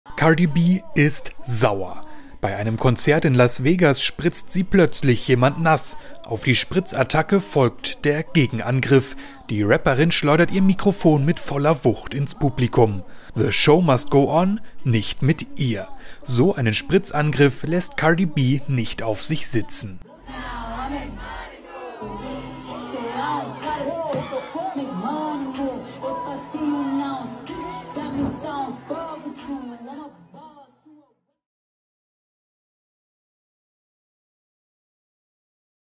Rapperin Cardi B wirft Mikrofon auf Fans: Ausraster nach Spritz-Attacke
Cardi B ist sauer! Bei einem Konzert in Las Vegas spritzt sie plötzlich jemand nass. Auf die Spritz-Attacke folgt der Gegenangriff: Die Rapperin schleudert ihr Mikrofon mit voller Wucht ins Publikum.